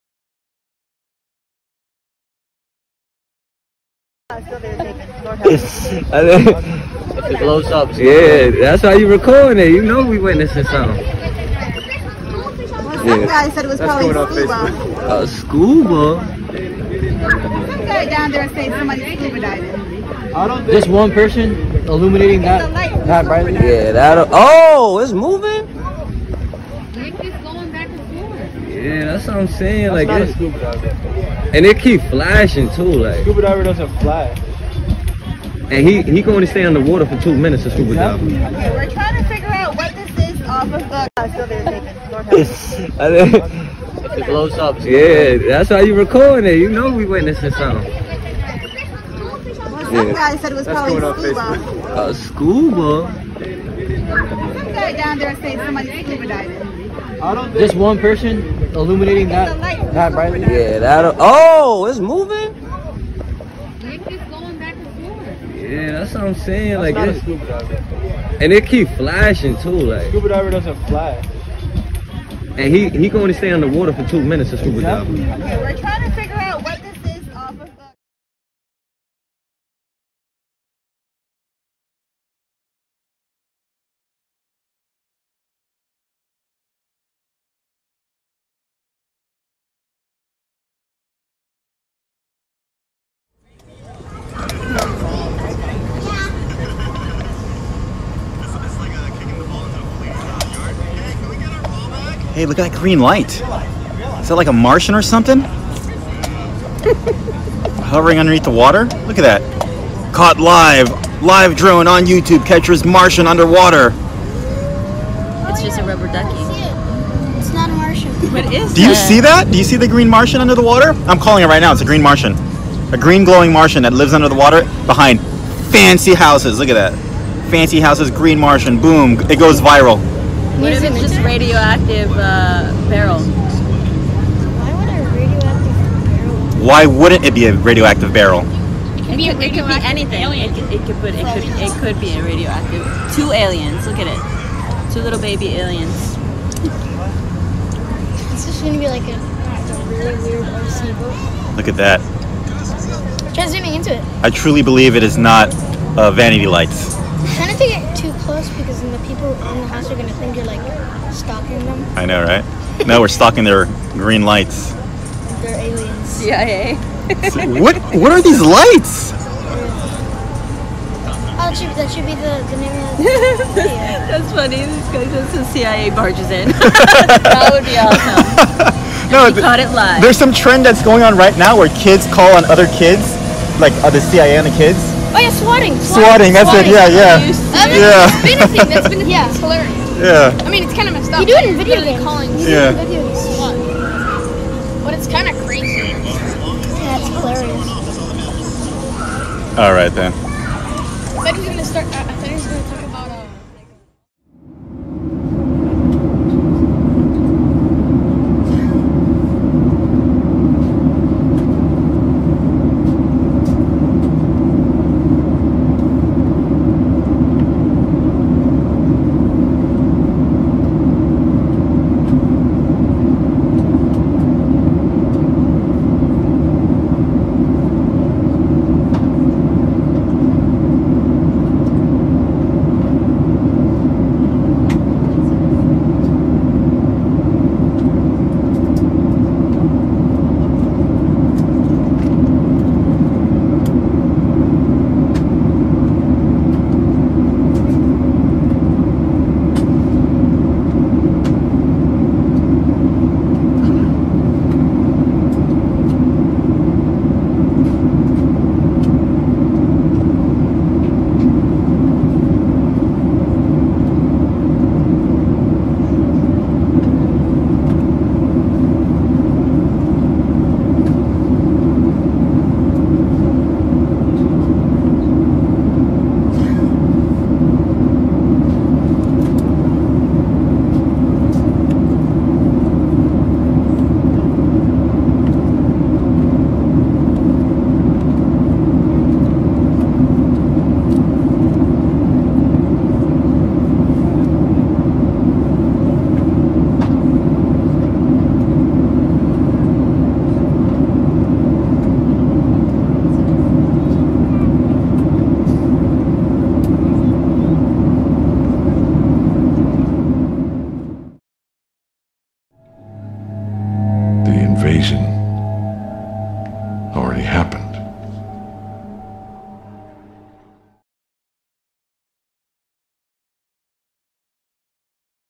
Luzes submarinas_Underwater lights. Miami, US / East Siberian Sea, Russia
UFO IN MIAMI BEACH WATER ?
29/05/2021
Credit: 59 Escobar
Miami, US
East Siberian Sea, Russia. 01/06/2020. Credit: Anonymous
Alien Lights Found Under Water Drone Footage Miami?
04/07/2018
Credit: Lo Knows Tech
Miami, US
East Siberian Sea Lights
01/06/2020
Credit: Testemunha anônima_Anonymous witness
Siberian Sea, Russia